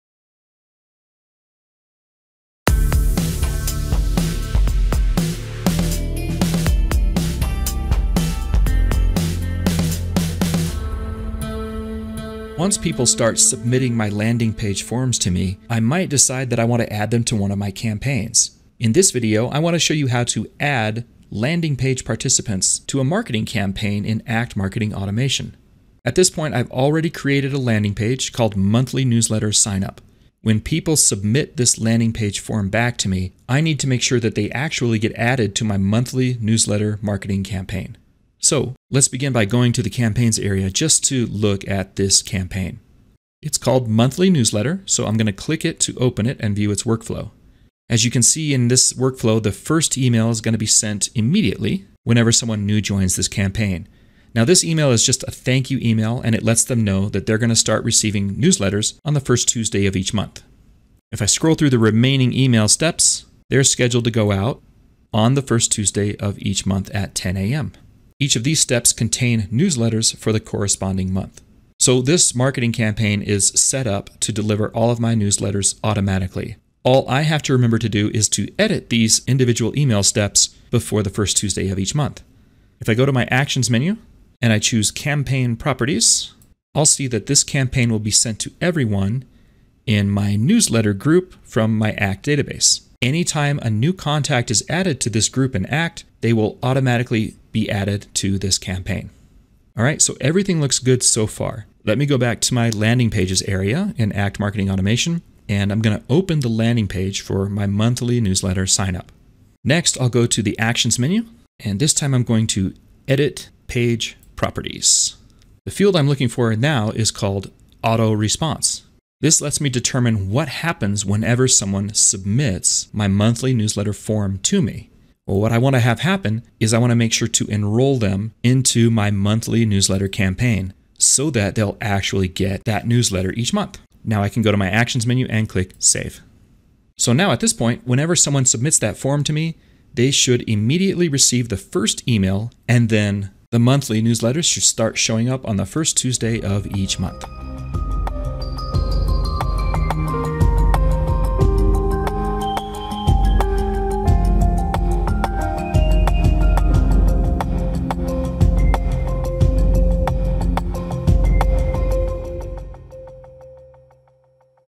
Act! Training Video - AMA: Add Participants to a Campaign
How to add Landing Page participants to a Marketing Campaign in Act! Marketing Automation